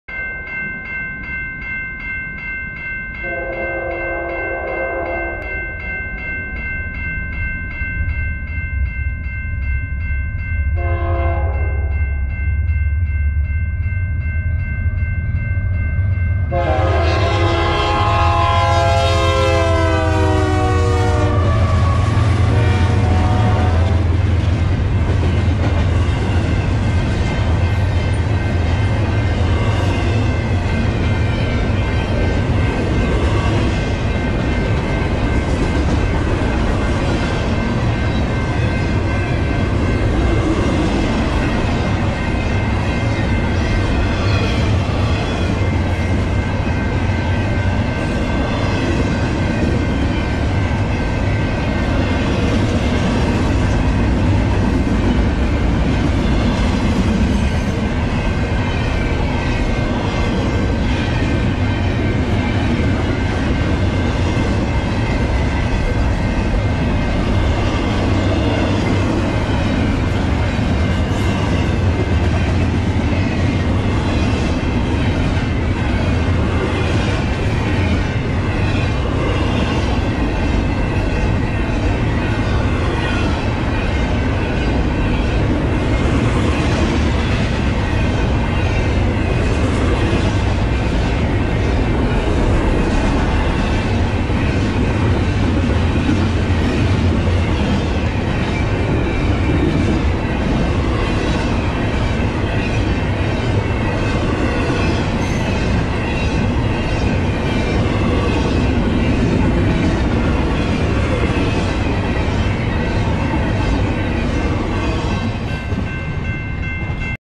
East Bound BNSF intermodal with an SD70Ace
BNSF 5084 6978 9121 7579 leads an east bound intermodal through Santa Fe Springs.